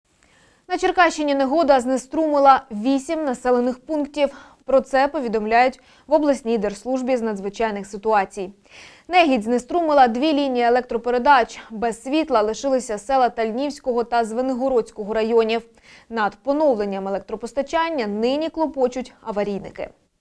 На Черкащині негода знеструмила вісім сіл
Негідь знеструмила 2 лінії електропередач